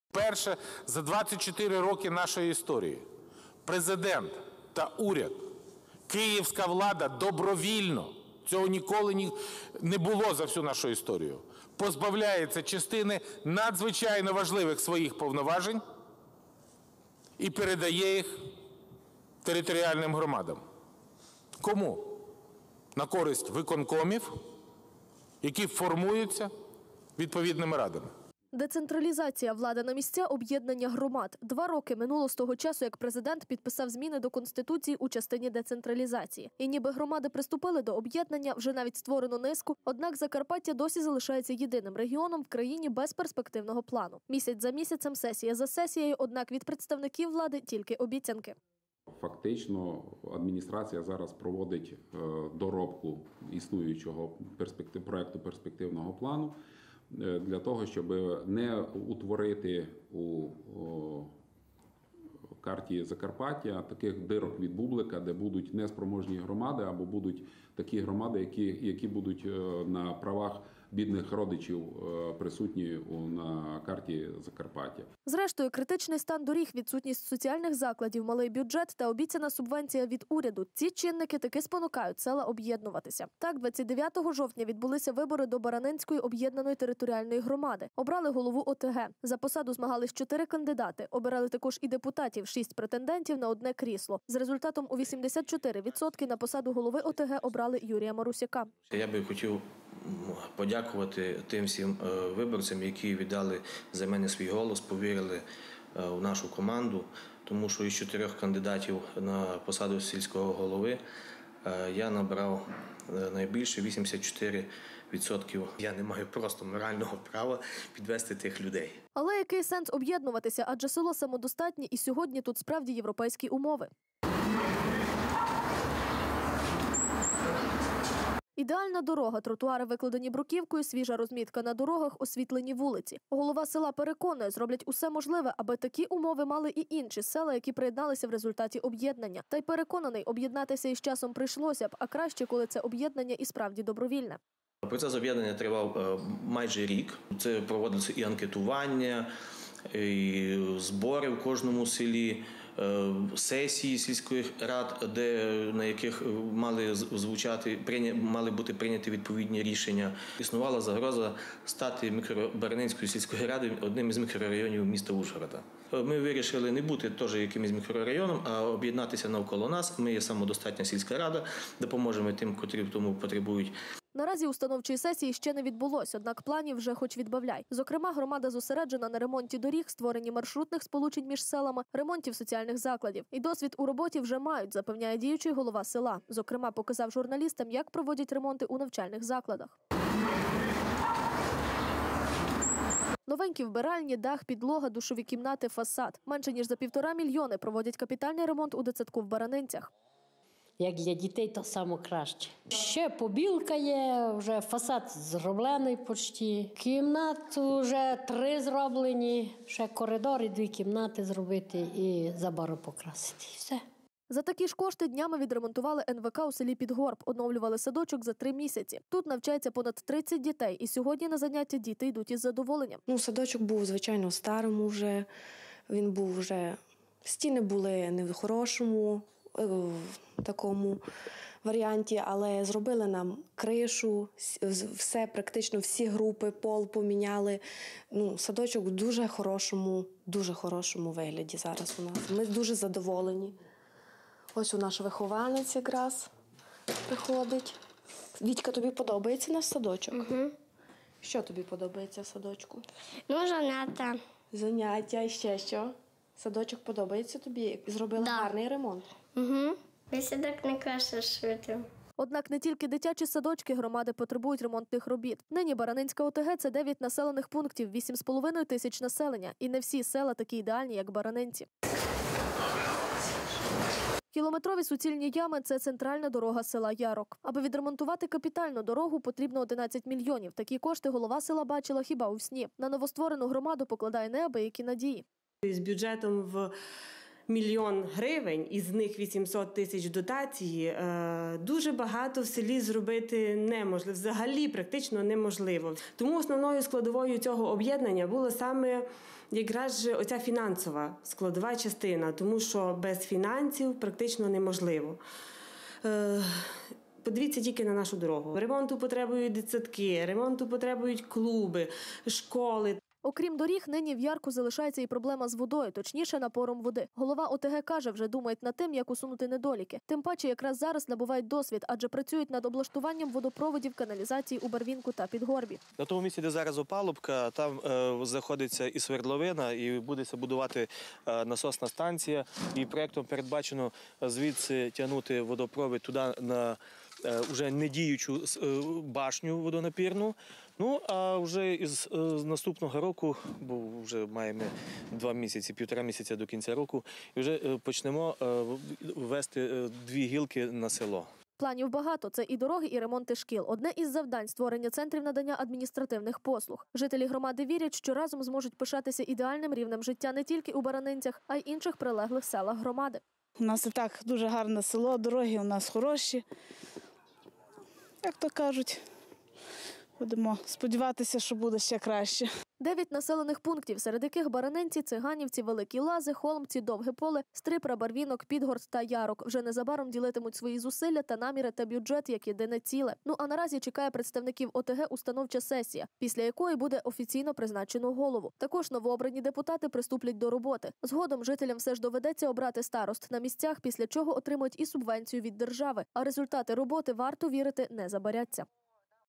Об’єдналися
Після виборів 29 жовтня в Україні об’єднано 665 громад, з яких 159 громад, які сформувалися в 2015 році – вже показали реальні успіхи в об’єднанні, відповідальності і реалізації перспективних проектів. Наразі 3,6 млн сільського населення вже проживає в ОТГ, ще очікує об’єднання в громади понад 9 мільйонів. В уряді закликають новообраних голів ОТГ підготувати перспективні проекти, які будуть реалізовані в кожній з громад. На Закарпатті запрацювала ще одна територіальна об’єднана громада. Цього разу ОТГ на Ужгородщині з центром у селі Баранинці. Вибори голови та депутатів відбулись 29 жовтня у 9-ти селах, що вирішили об’єднатись